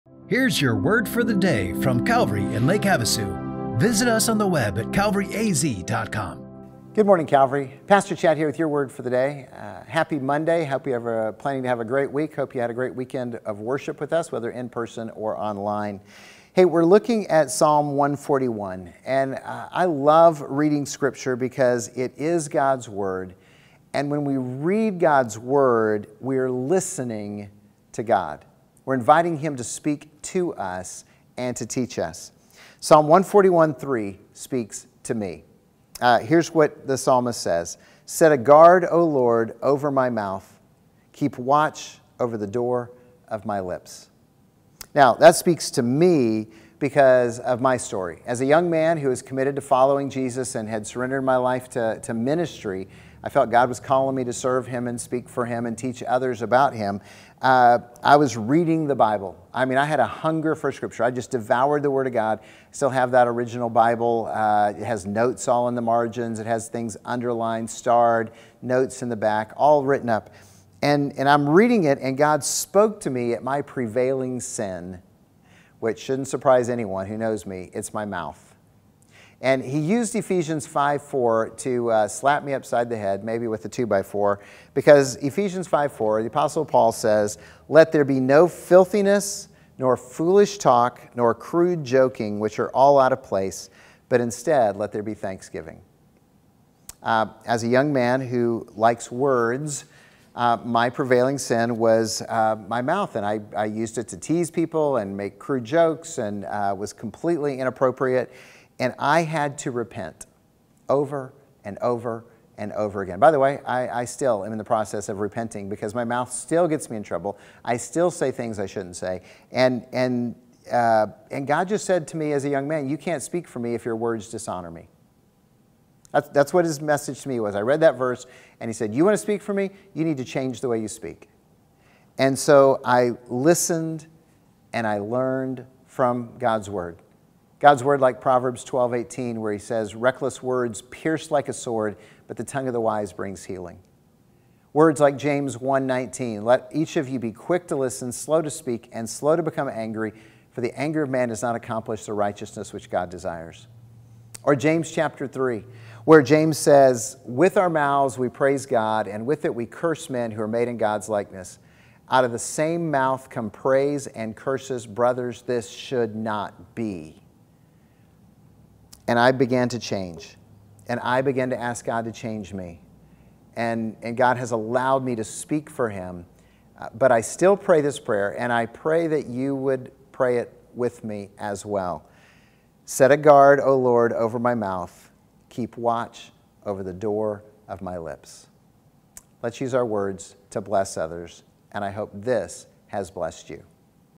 Your Word for the Day - Episode 870
Today Pastor Chad prompts us to ask God to guide our words to avoid filthy talk, foolishness, or cruel joking.